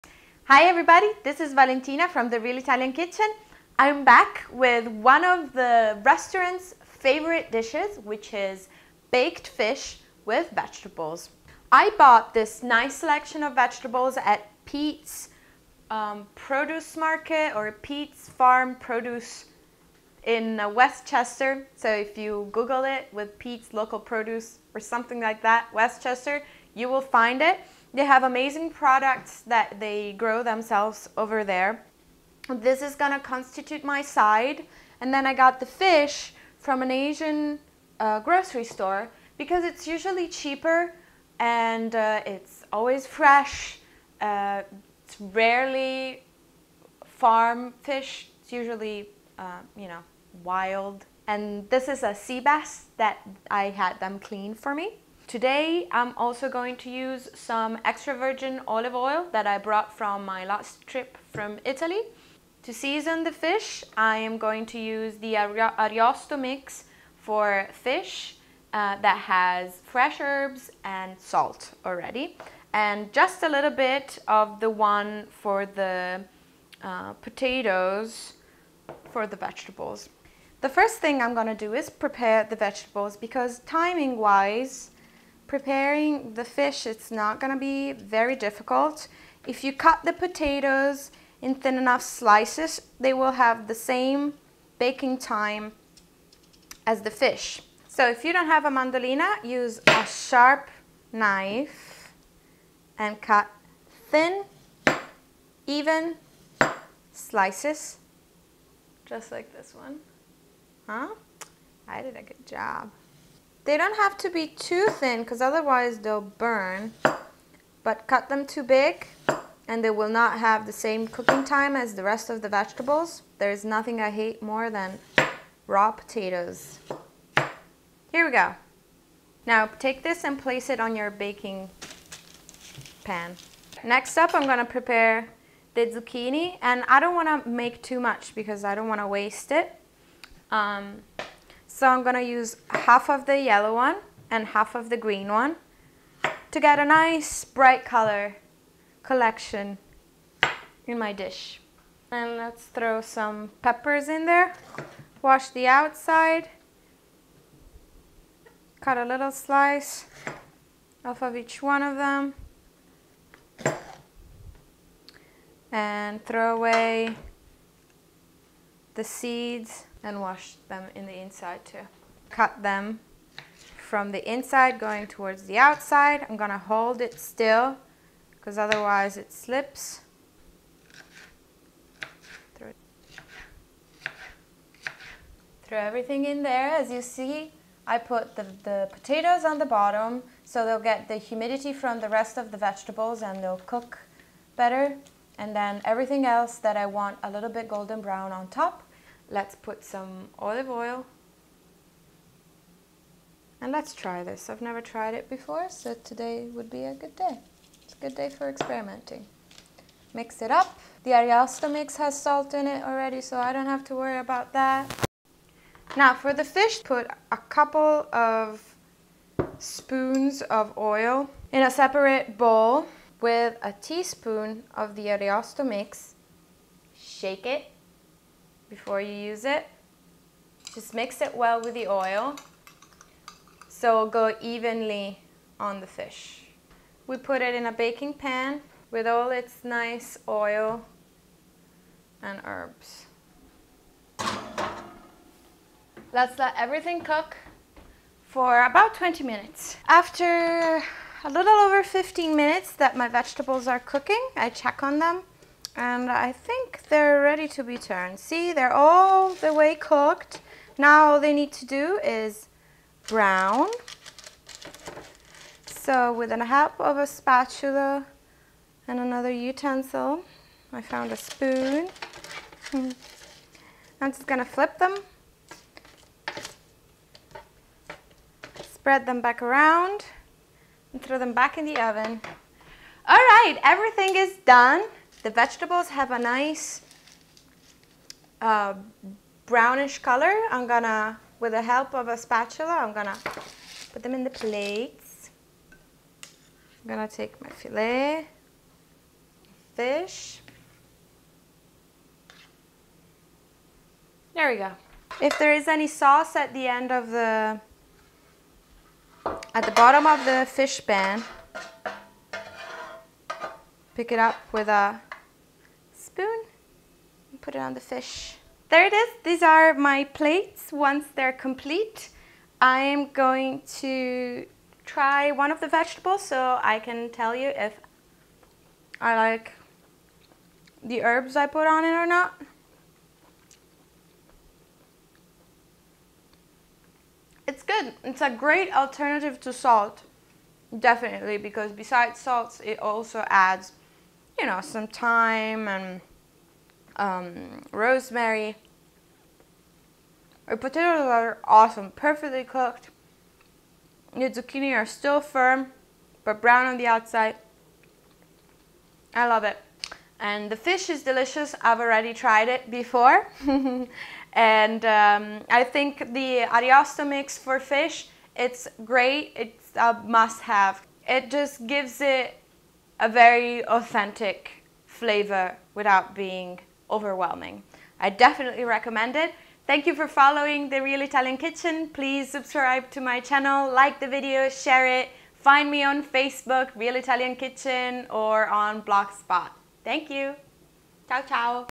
Baked Sea Bass with Vegetables - Real Italian Kitchen
Simple, fast, complete meal for a great dinner idea.
I'm not endorsed by any of these companies, I use their products because I like them.